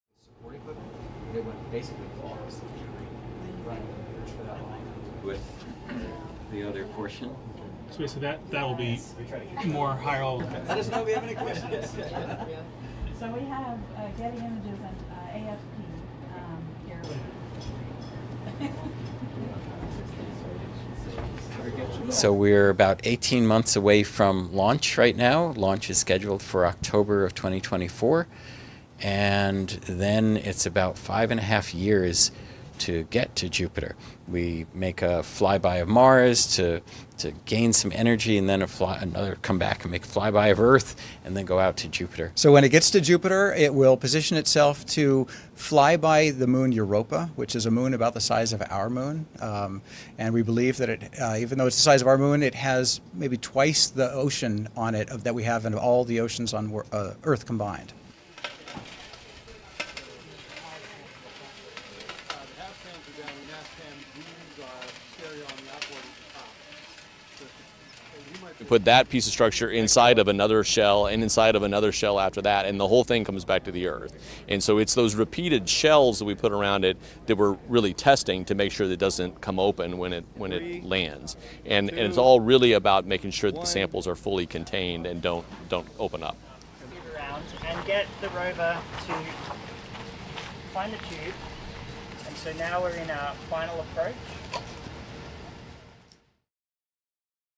La Nasa ouvre son laboratoire pour présenter ses missions sur Mars et Jupiter | AFP
Au laboratoire Jet Propulsion, à Pasadena, près de Los Angeles, la Nasa a ouvert son laboratoire pour présenter sa mission sur Mars et Jupiter.